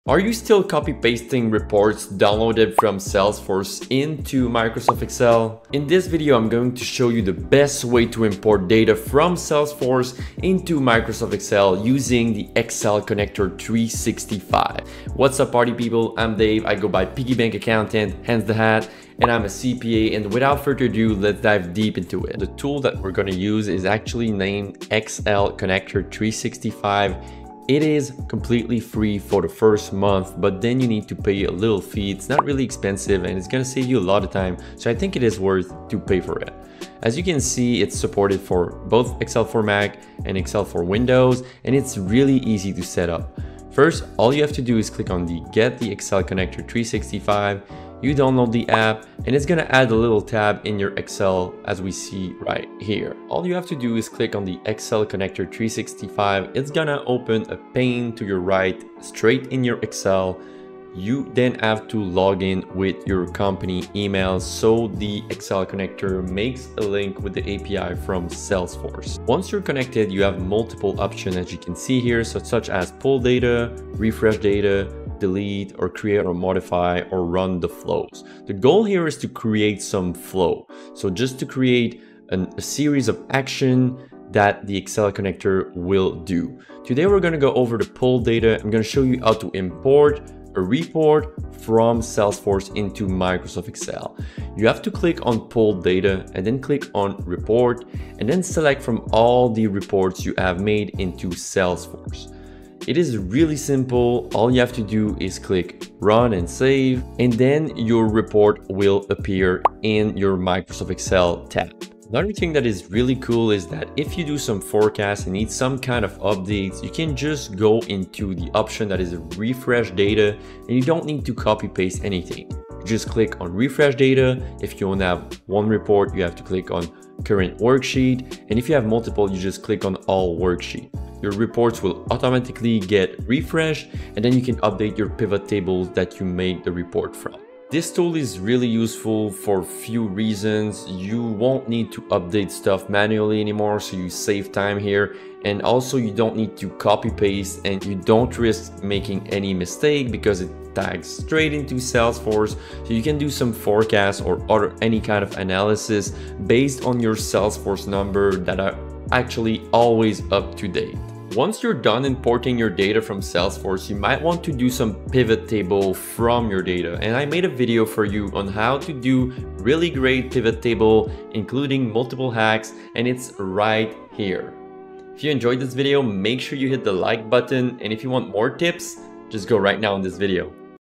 How To Connect Salesforce With Microsoft Excel
In this video, I'll show you how to connect Salesforce with Microsoft Excel, importing reports from both and vice-versa, thus improving your efficiency and reducing mistakes.

Download Your FREE Copilot Cheat Sheet Here!

Copilot Essentials Course: From AI Beginner to Proficient is now available➡️